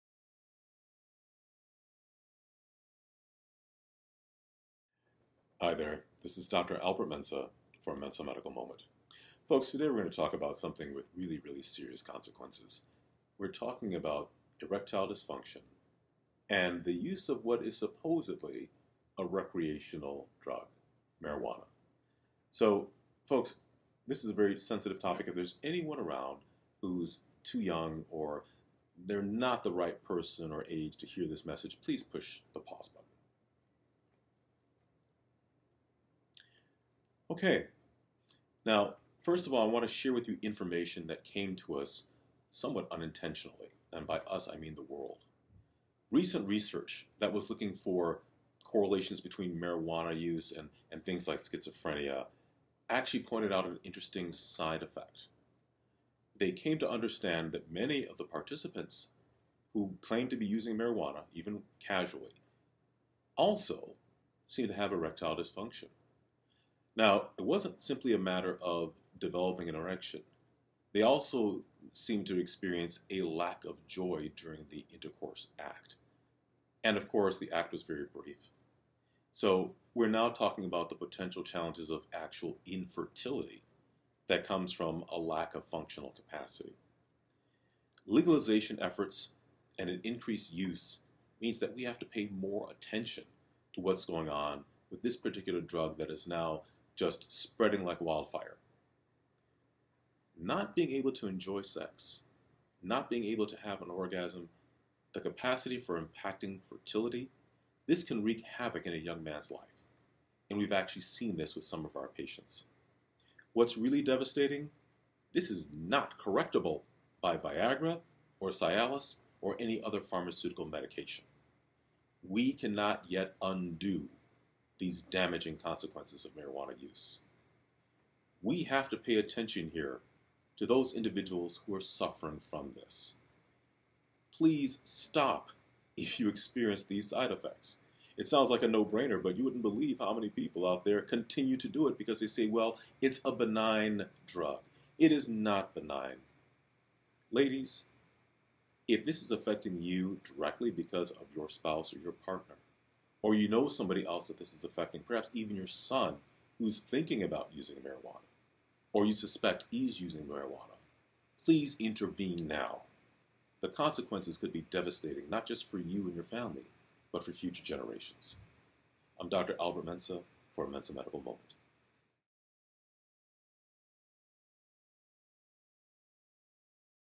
MMM 7 Weed & ED
In this "Mensah Medical Moment," Dr. Albert Mensah recounts recent studies that show a link between marijuana use and Erectile Disfunction.